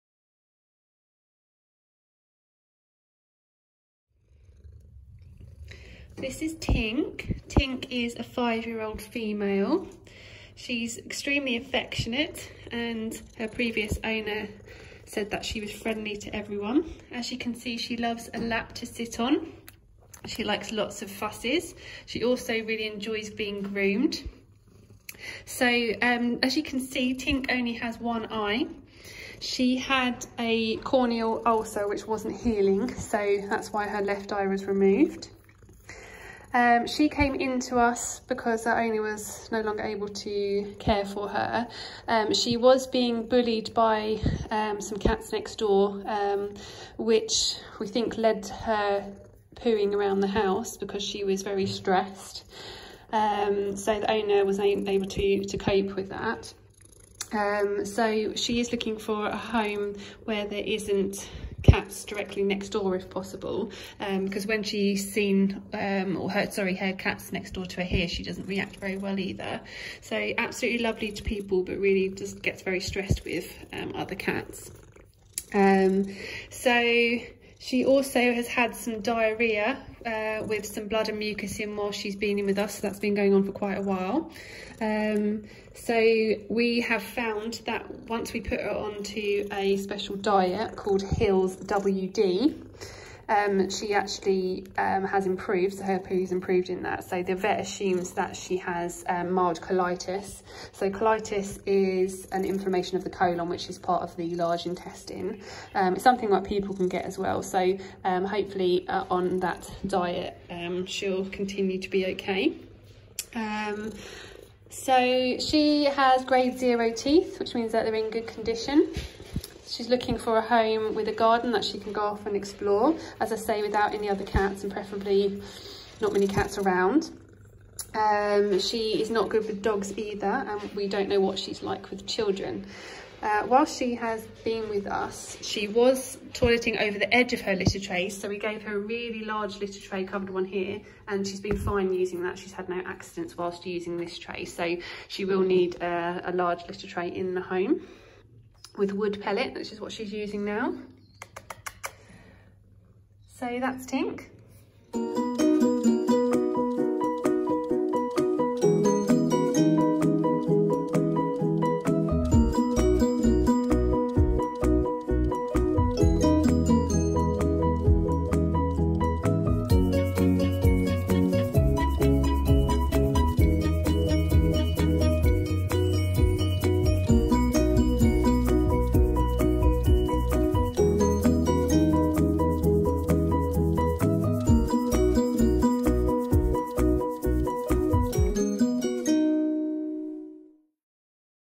Tink 490652 Homing Video
Tink has been lucky enough to have been reserved but please do visit our and see other cats that we have in our care looking for their forever homes.